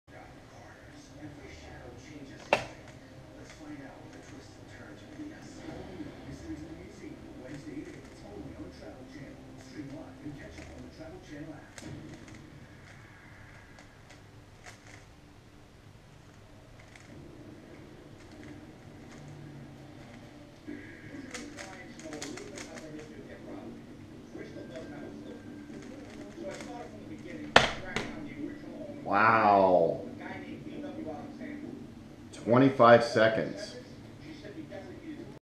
3x3 solve - 25.07 seconds
First solve of the night, and everything fell into place.  Not sure why I have a hard time recognizing J Perm right away.